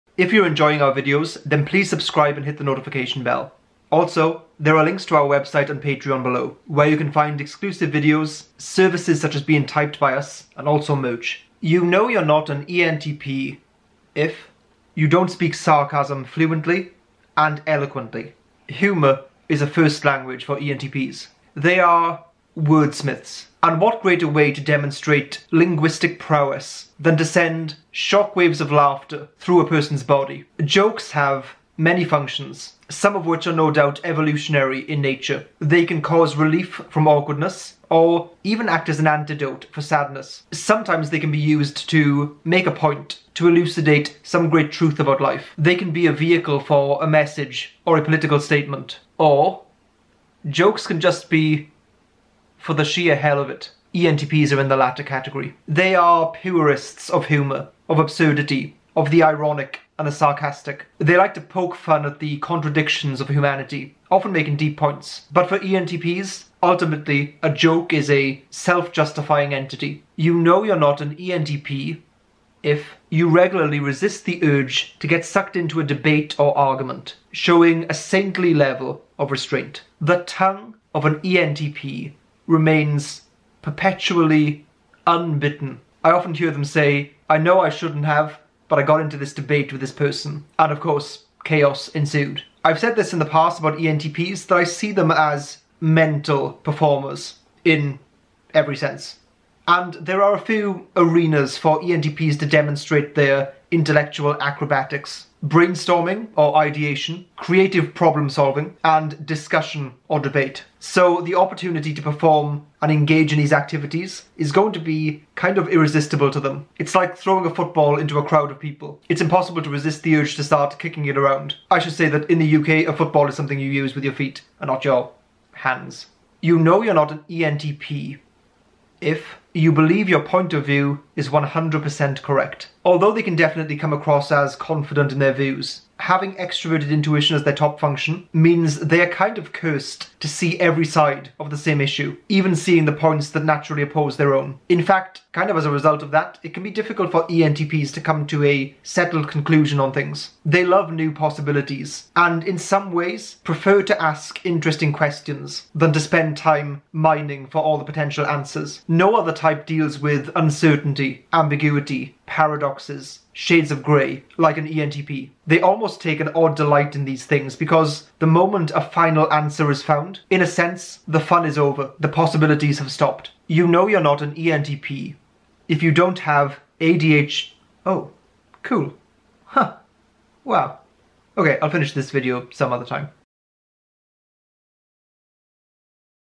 You know you're NOT an ENTP when...
Are you really an innovator, an instigator, an alligator ok wait that one doesn't make sense... well let's find whether you're an ENTP shall we?

Luann Snapchat - @luknepp
Luann Instagram - @luannknepp